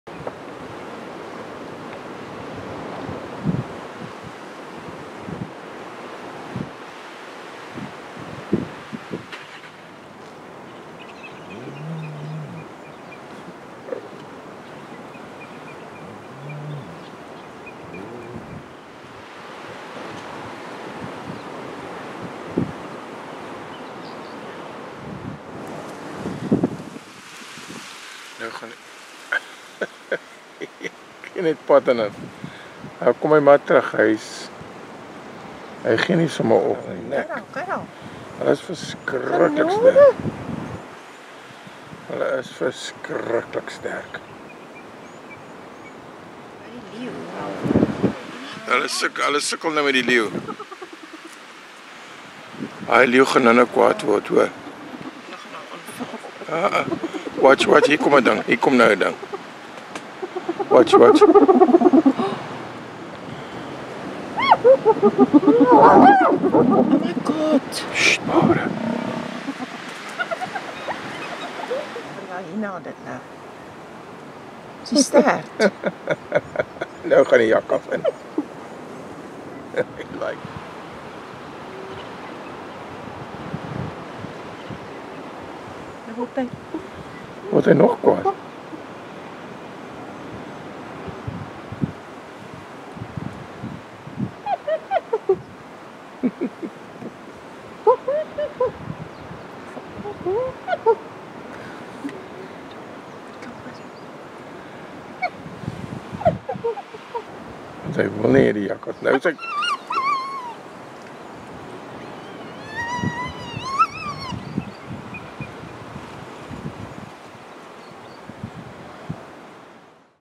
Hyenas, jackals and lion argue as to who the owner of the blue wildebeest carcass is
Dominant hyena claims ownership of a blue wildebeest carcass and keeps the clan and jackals at bay. Hyena shows it's strength when dragging carcass. Frustrated clan turns attention to a lioness (likely the real owner). Dominant hyena then claims the carcass tail, drops it when chasing jackal and loses it to a young clan member.

African Eye wildlife